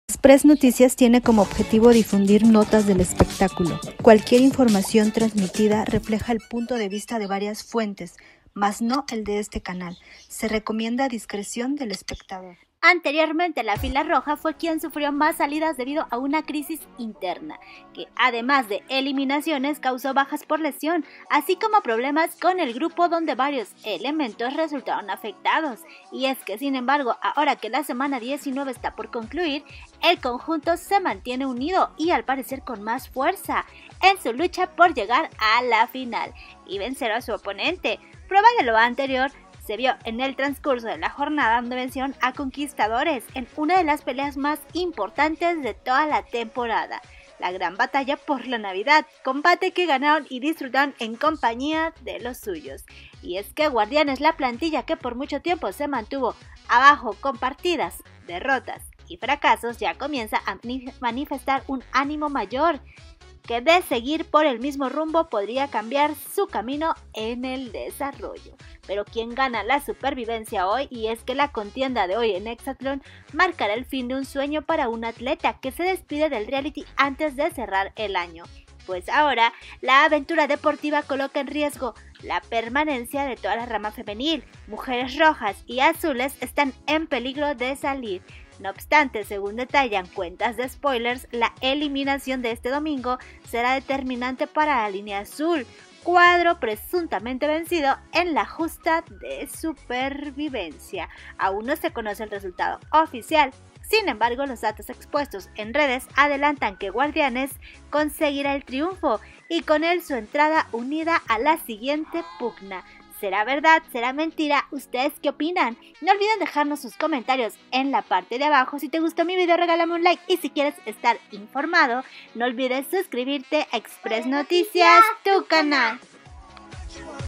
¿Quién gana la Supervivencia HOY domingo 26 de diciembre en Exatlón México?
Mujeres rojas y azules corren esta noche en Exatlón México por salvar su estancia en la famosa contienda deportiva.UNETE A NUESTRO CANAL HERMANO